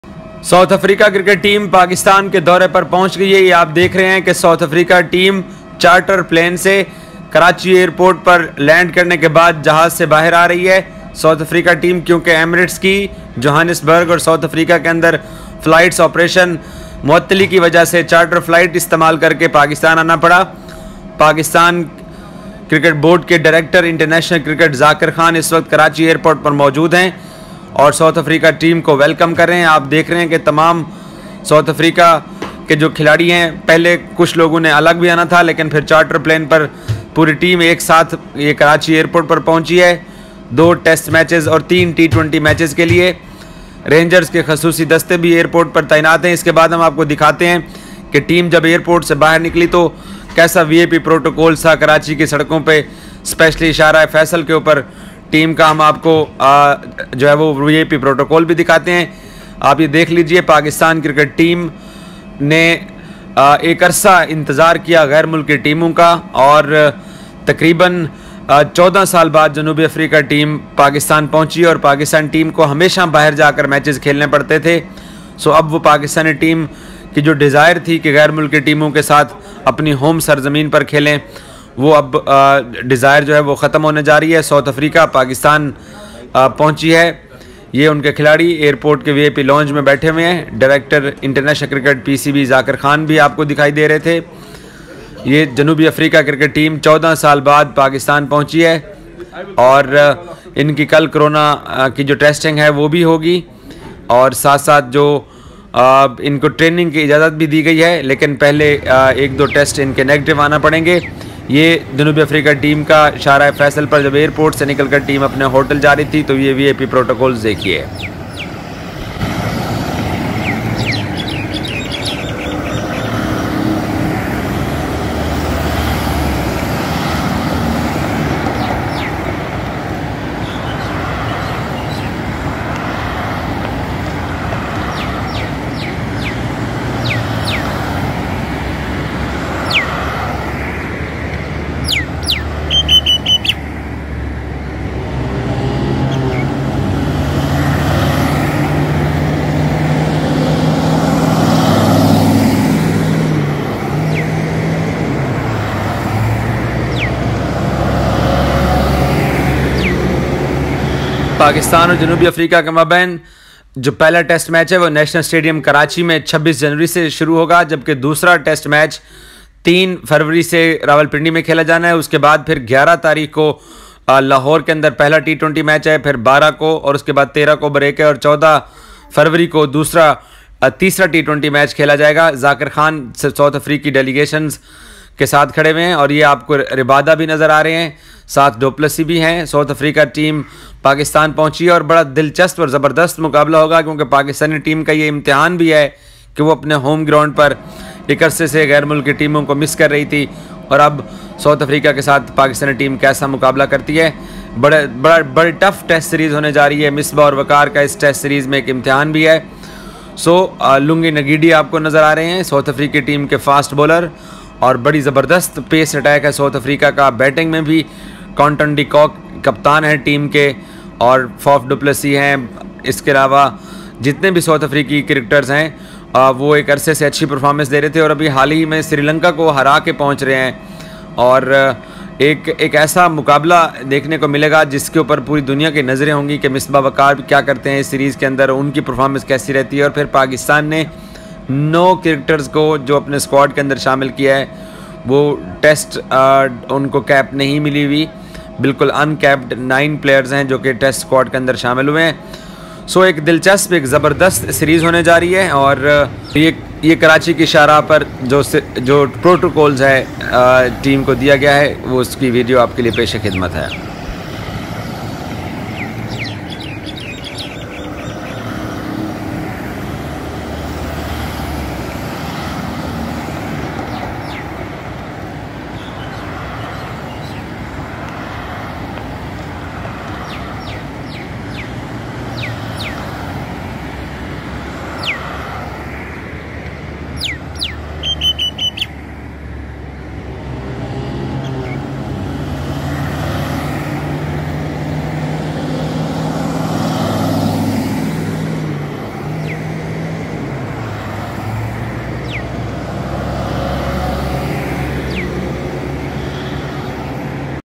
VIP protocol South Africa Team Reached Karachi Pakistan | SA VS PAK TEST SERIES | Proteas cricket
South Africa Cricket team Reached Karachi Pakistan

South Africa Cricket Team arrives in Karachi Pakistan

south Africa tour of Pakistan